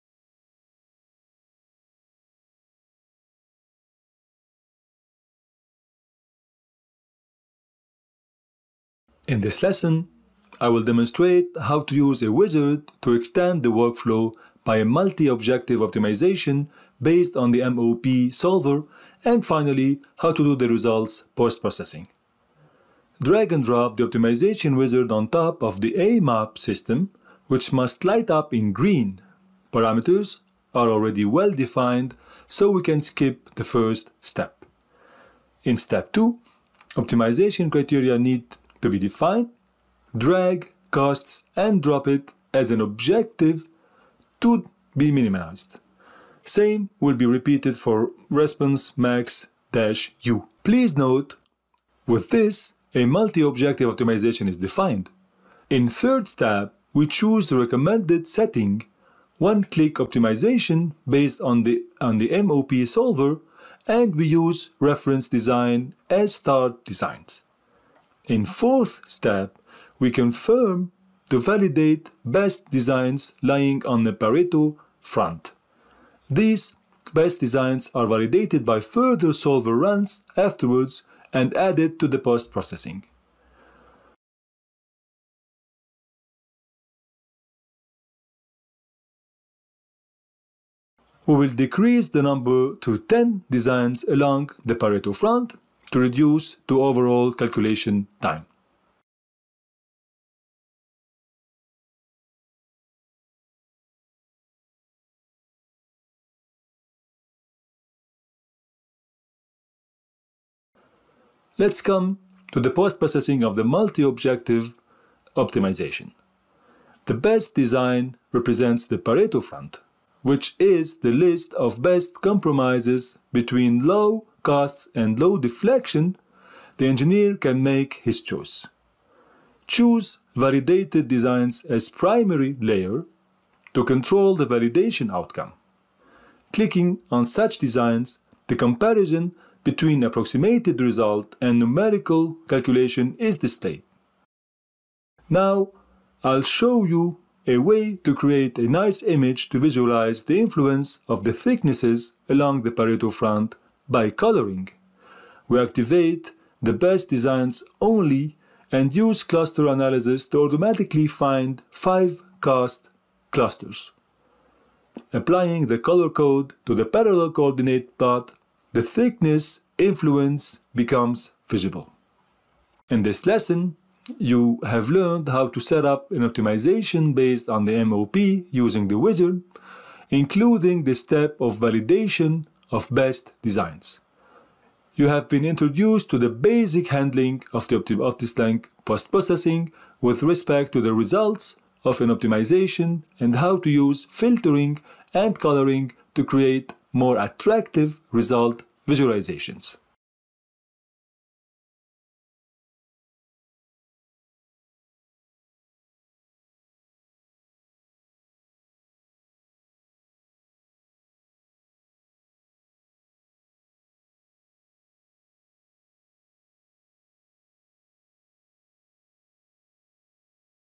Multi-Objective Optimization – Lesson 6
Once again, a wizard is used to easily set up a multi-objective optimization.
Due to its very good prediction quality, the Adaptive Metamodel of Optimal Prognosis (AMOP) can be used to approximate the responses with sufficient accuracy. Thus, the optimization process can be performed in a much shorter time compared to time-consuming numerical simulations. Nevertheless, it will be shown how best designs along the Pareto-Front can be validated.
Furthermore, it will be shown how to easily evaluate and comprehensibly visualize the results in the Postprocessing.

Chapters -
00:09 Start using the Optimization wizard
00:36 Step 2: Define optimization criteria
00:57 Step 3: Choose the optimization method
01:08 Step 4: Additional optimization options
01:31 Reducing the number of Pareto designs to be validated
01:51 Multi-objective optimization postprocessing
02:08 Control validated design
02:35 Cluster analysis to divide Pareto front by color
02:52 Summary (audio)